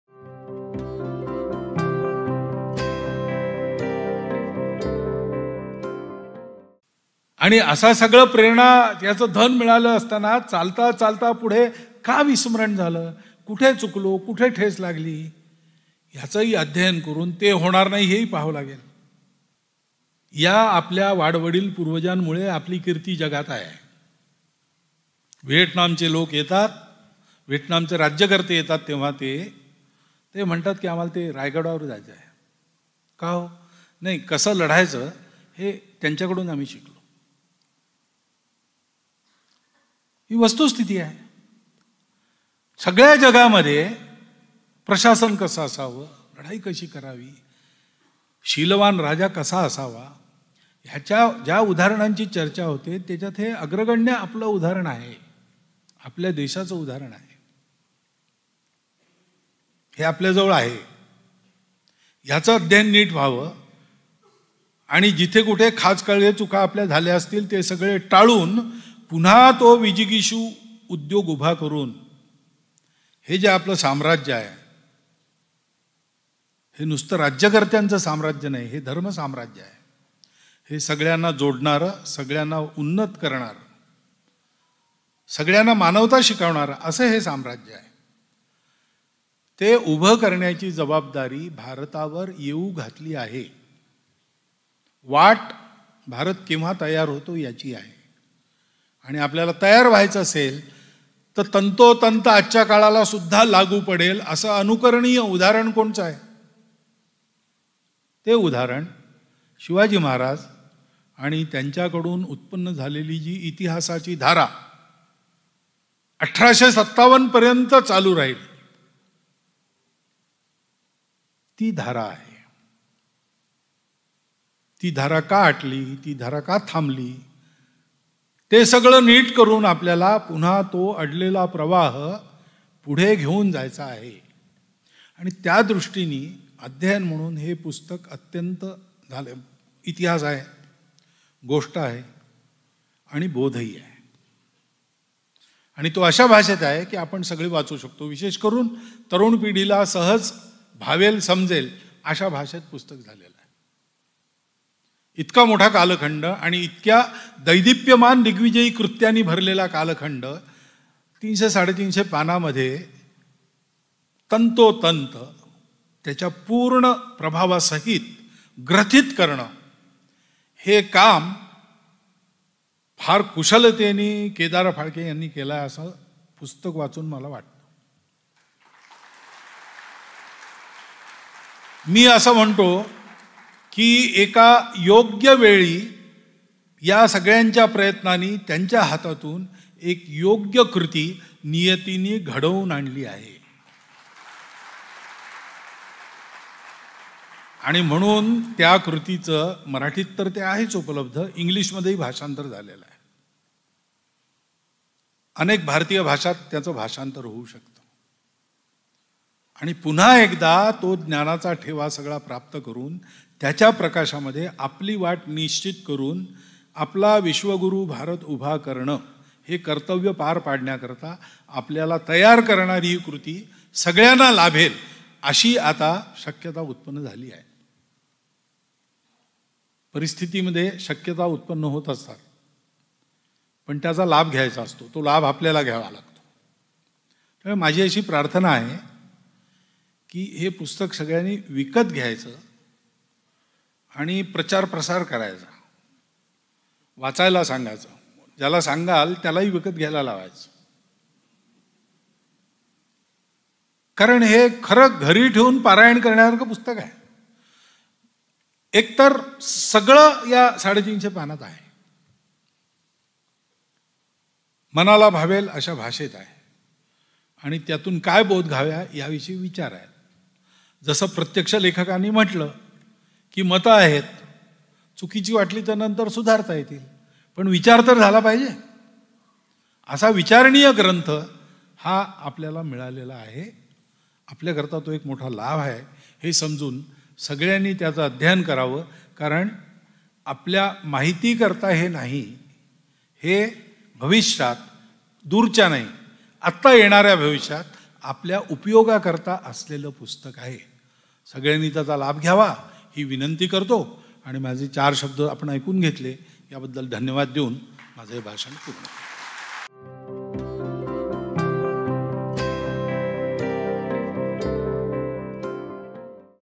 The Legacy Of Chhatrapati Shivaji - an important book for our near future
An excerpt of Dr. Mohan ji Bhagwat's speech at the book launch of the Marathi book :
" शिवछत्रपतींचा वारसा, स्वराज्य ते साम्राज्य "
 - लेखक : डाॅ. केदार फाळके

The English translation of the book  : " The Legacy Of Chhatrapati Shivaji – From Kingdom To Empire "  by Dr. Kedar Phalke.

Both books are available on Hindu eShop :
Quick link :
Marathi Original :